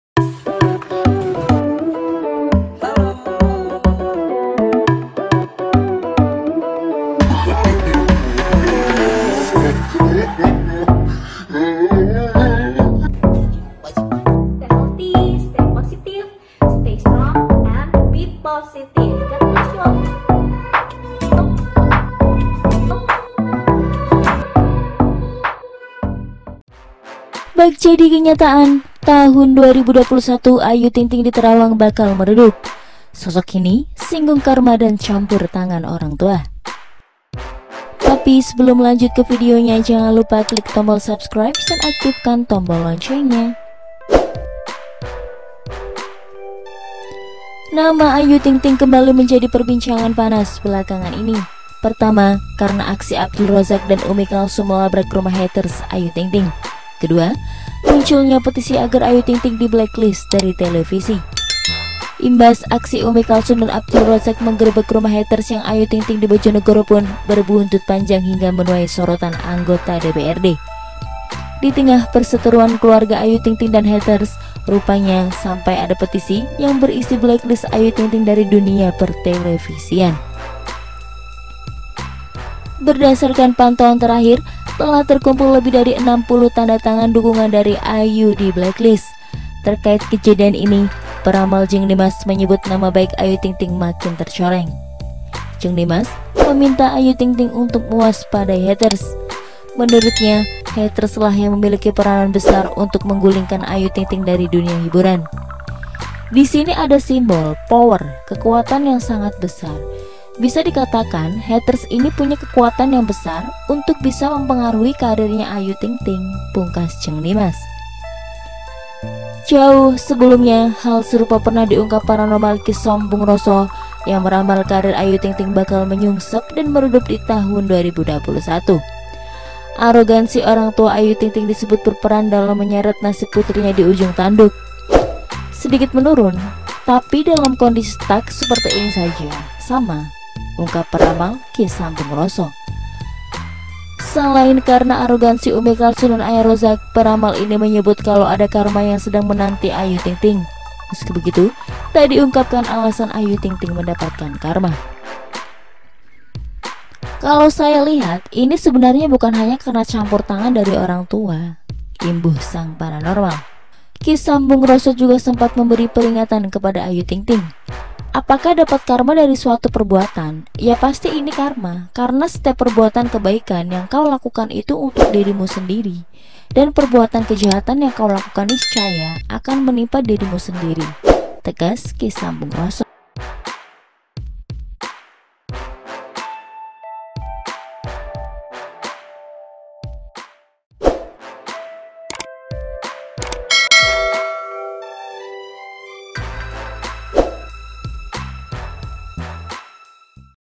Bak jadi Kenyataan, Tahun 2021 Ayu Ting Ting Diterawang Bakal Meredup, Sosok Ini Singgung Karma
Tonton Juga:
Tak Hanya Ayu Ting Ting, Artis Ini Ternyata Juga Pernah Dapat Petisi Pemboikotan Gegara Hal Ini